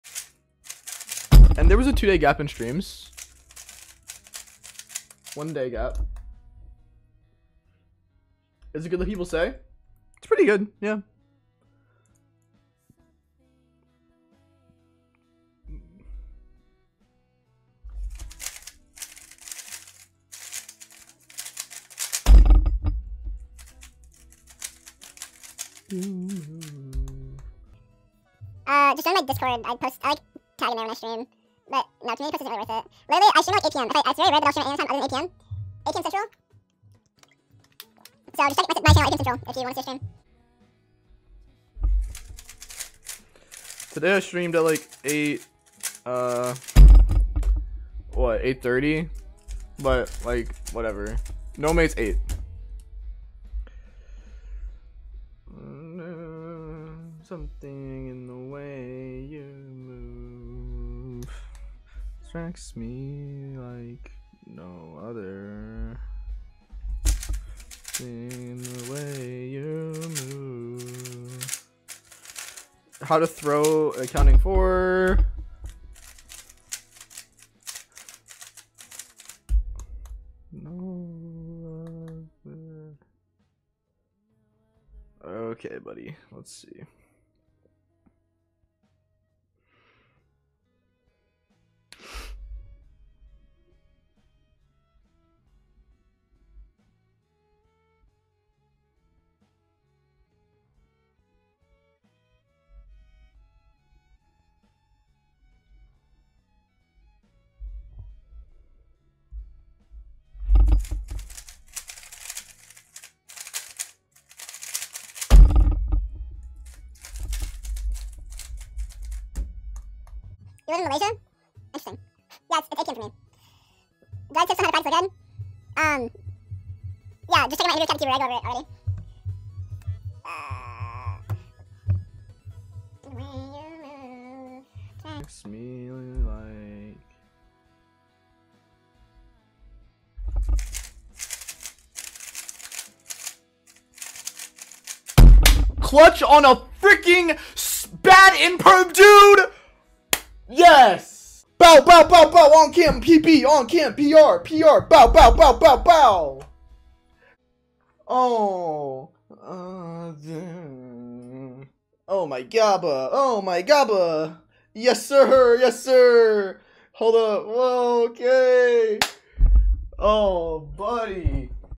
4.89 3x3 Average!
I may have overstated the clutch factor here by not acknowledging the oll skip but regardless pretty clean ao5 through and through, no skips, stackmat, all counting 4’s, pretty consistent, and clutch after throwing solve 3. Feel like i’ve been improving for sure, even got another sub 5 average and 2 sub 5.1 averages in this stream as well. Been nice. Hope you enjoyed the video and if you were there live please watch the whole thing and comment if you were there ty :P

I also stream on youtube!

Time List:
1. (4.31)   U2 R' D B' D2 L B L2 U2 L2 U B2 U B2 D2 F2 U' B2 D L
2. 4.89   F U' F' B' D B' D2 L B F2 D2 L D2 B2 R' F2 L U2 R F2
3. (7.38)   F' U F' R D B2 U' B2 R2 U2 D2 R F2 D2 R L B2 U F2
4. 4.87   L' F U D2 B' U D2 L' F' B2 R2 L2 D2 R2 B2 U L2 B2 D R2 F2
5. 4.90   U2 B L2 F2 D2 R2 B L2 B L2 D' L' D2 U R2 B' F D' R2